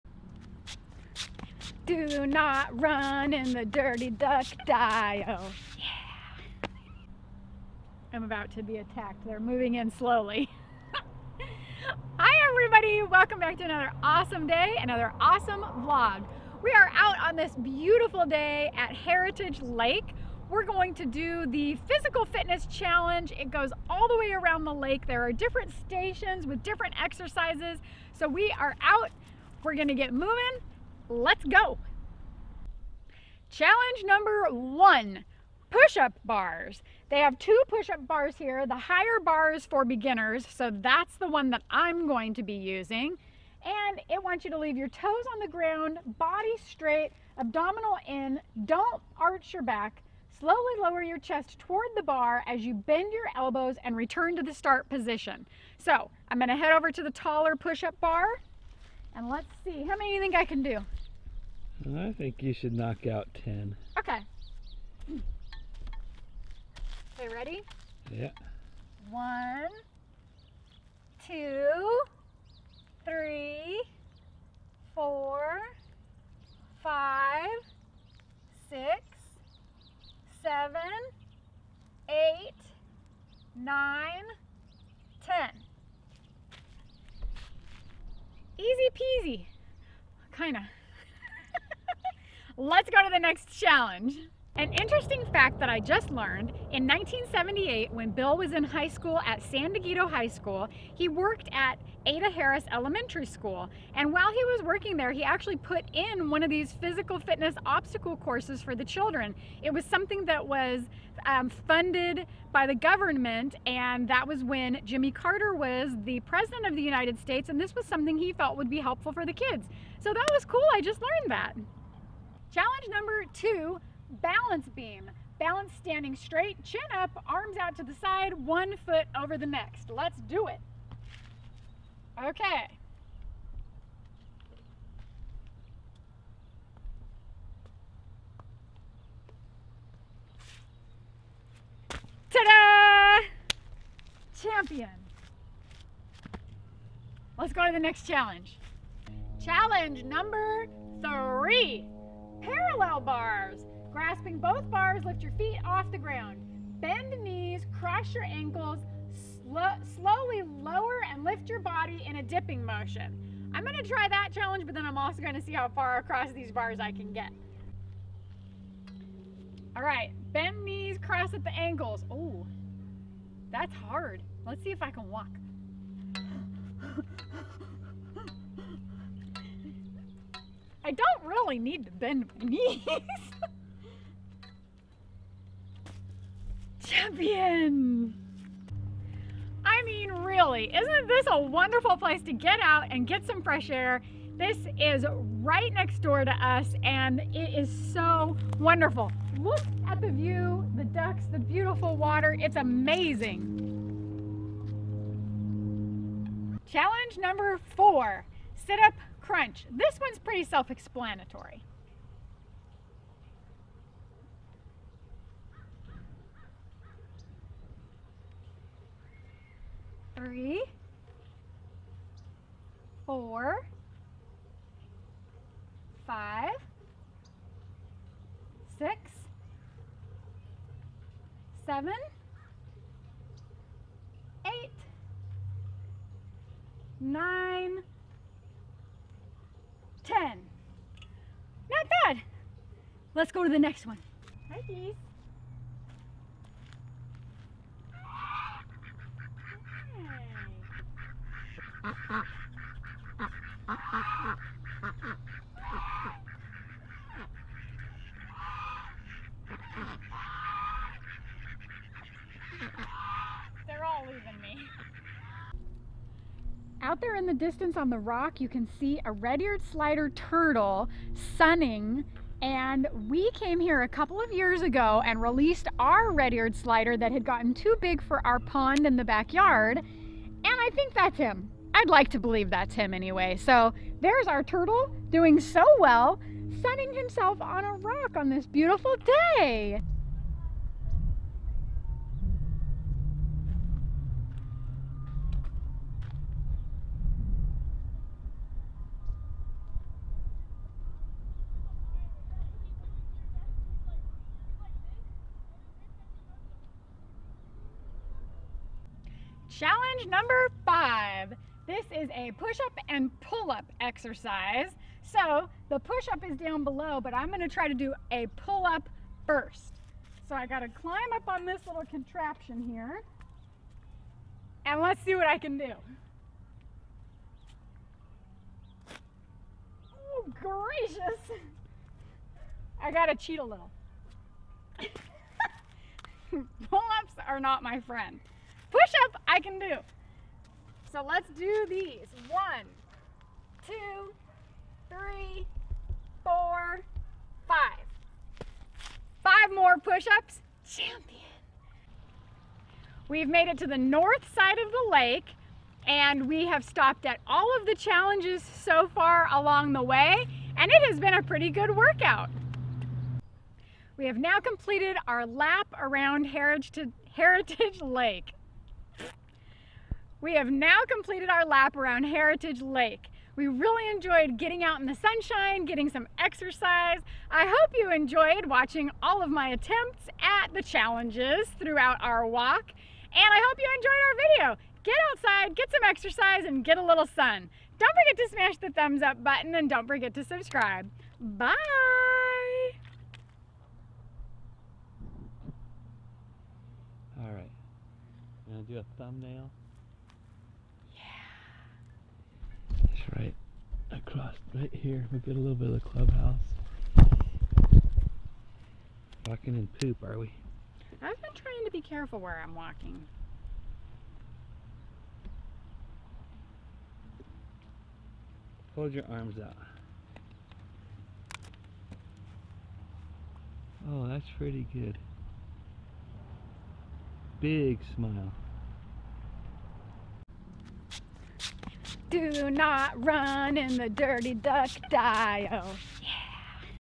Heritage Lake menifee California. Beautiful Walking Trail And Excercise Stations 25 Acre Lake
On this beautiful day we went to our near by neighborhood Heritage Lake. This is a 25 acre lake with a concrete walking trail around it. There are exercise stations along the way. Can Lisa complete the tasks or just get lost in playing with the Geese, birds, ducks and turtles? Anyway, What a peaceful couple of hours outdoors. Have an awesome day, Billisa